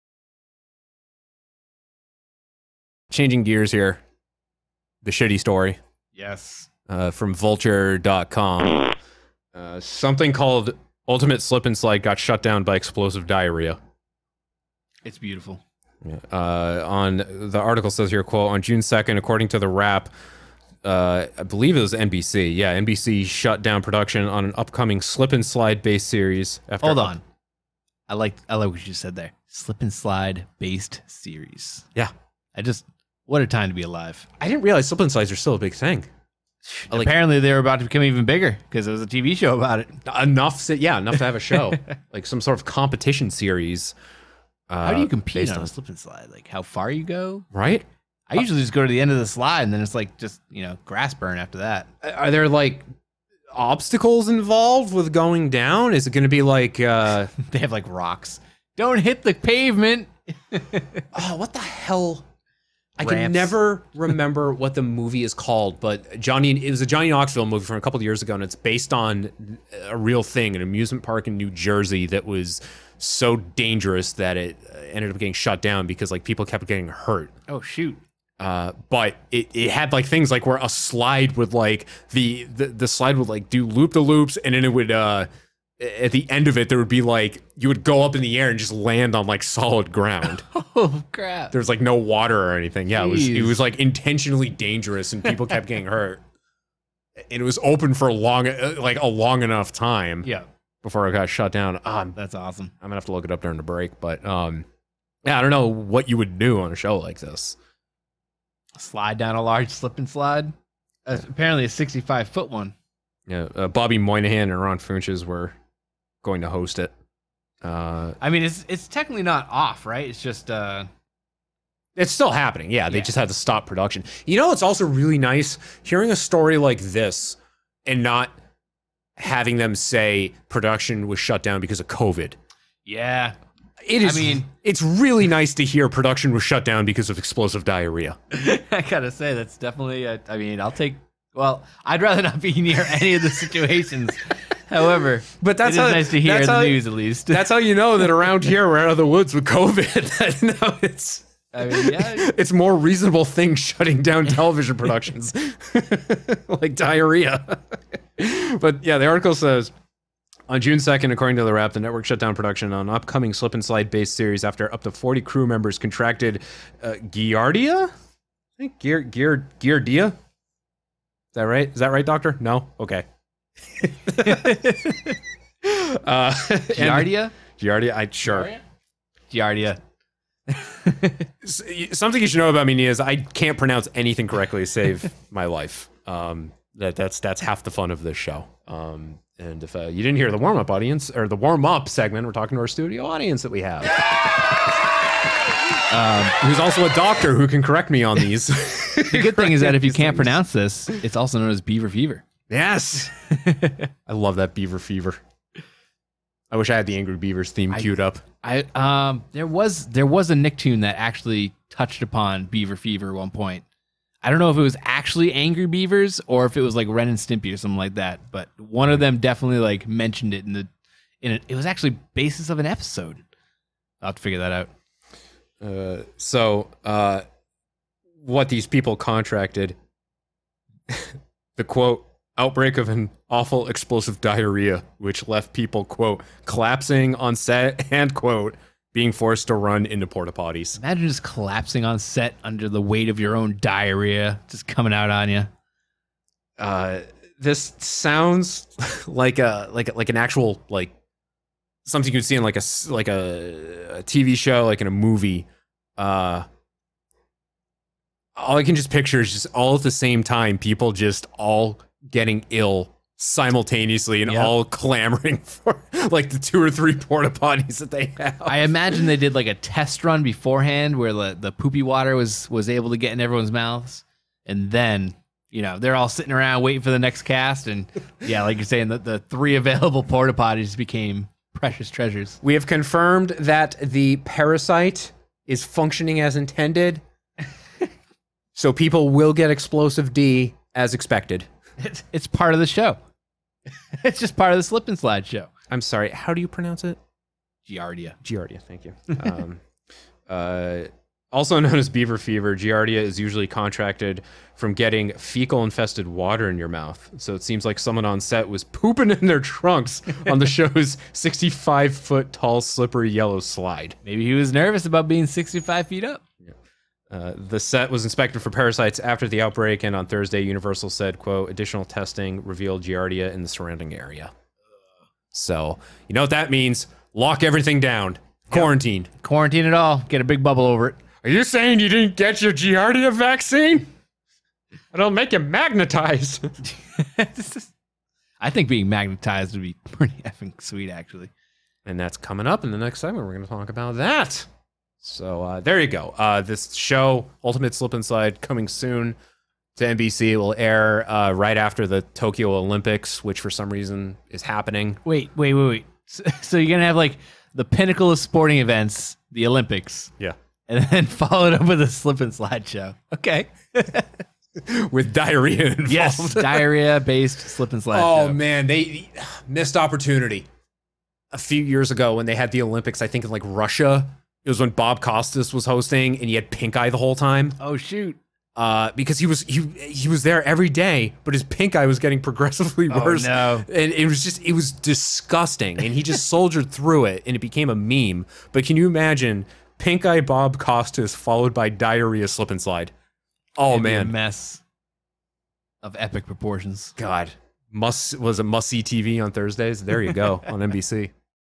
Ultimate Slip 'n Slide production halted due to "severe diarrhea"
In this segment from the 503rd episode of Hardly Focused, Jack and Fro discuss the upcoming NBC show "Ultimate Slip 'n Slide," the production of which came to a halt following an outbreak of "severe diarrhea."

This podcast contains explicit content. Anything you hear should not be taken seriously. Views and opinions expressed by the hosts of this podcast are their own.